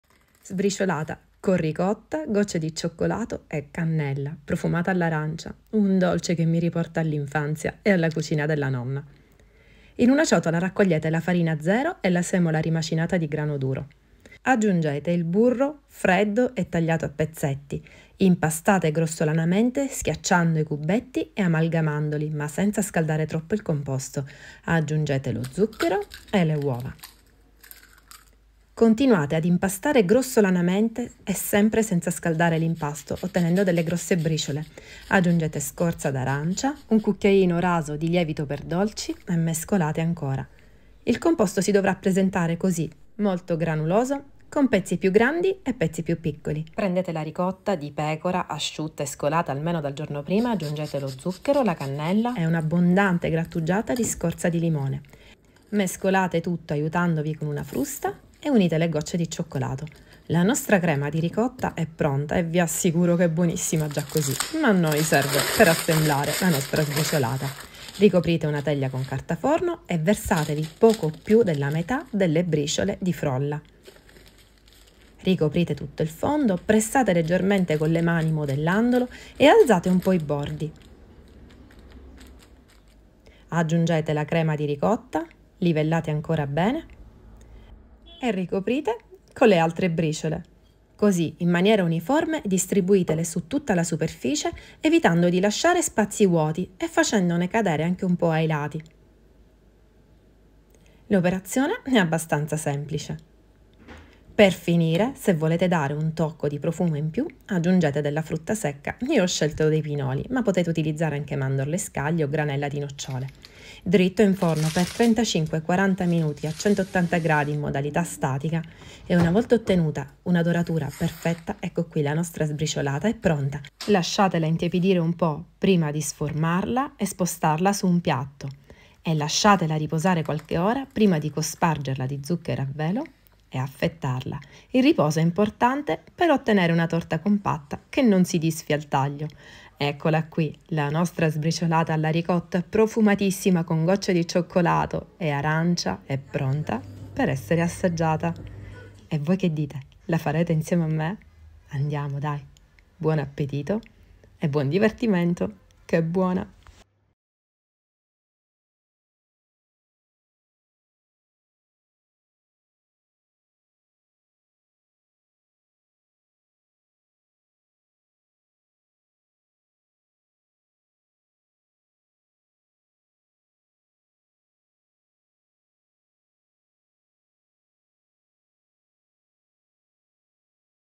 Sbriciolata alla ricotta
Sbriciolata alla ricotta con gocce di cioccolato.
Sbriciolata alla ricotta e cioccolata, la ricetta della nonna per una sbriciolata profumatissima con un ripieno cremoso e morbido, un dolce perfetto quando cominciano i primi freddi e ci fa compagnia fino a primavera inoltrata.
Ingredienti per fare la frolla della sbriciolata
200 g farina 00
100 g semola rimacinata di grano duro
150 g burro
2 uova
130 g zucchero
1 cucchiaino lievito chimico in polvere (raso)
q.b. pinoli
q.b. scorza d’arancia
q.b. zucchero a velo
Ingredienti per il ripieno di ricotta e gocce di cioccolato
600 g ricotta di pecora (ben scolata)
60 g zucchero
1 cucchiaino cannella in polvere
q.b. scorza d’arancia
1 cucchiaino essenza di vaniglia
60 g gocce di cioccolato.
In una ciotola versare le due farine mescolate insieme, unite il burro freddo a pezzetti e lavorate grossolanamente.

Aggiungete lo zucchero, le uova e impastate velocemente. Aggiungete la scorza d’arancia e il lievito e riprendete a lavorare grossolanamente con la mano, fino a quando non vedrete formarsi le delle briciole di diverse dimensioni. L‘impasto è pronto, non andate altre, altrimenti il burro si scioglierà e otterrete una frolla classica.
Prendete la ricotta, deve essere ben scolata e almeno del giorno prima, raccoglietela in una ciotola insieme allo zucchero, alla cannella, alla scorza d’arancia e all’essenza di vaniglia, amalgamate e finite aggiungendo le gocce di cioccolato.
Ricoprite la base di uno stampo a cerniera da 22cm con carta forno, versatevi un po’ più di metà dell’impasto e con la mano modellatelo leggermente, alzando i bordi. Versate sulla base della sbriciolata la  crema di ricotta e livellate.
Coprire con la parte d’impasto rimasta, cuocere la sbriciolata alla ricotta in forno statico, preriscaldato a 180 per circa 35-40 minuti, ma regolatevi in base ai vostri forni, perché non tutti i forni sono uguali.
Quando la sbriciolata risulterà cotta e dorata, sfornatela, lasciatela intiepidire 40 minuti almeno e spostatela su una gratella. Lasciate riposare 5-6 ore prima di cospargete di zucchero a velo e servire.
La sbriciolata alla ricotta si conserva per 2 giorni a temperatura ambiente se non fa caldo e coperta da un canovaccio di cotone, altrimenti in frigo coperta da pellicola fino a 4giorni.

La ricotta di pecora da noi è salata, quindi se la vostra non lo fosse, aggiungete un pizzico di sale alla crema di ricotta.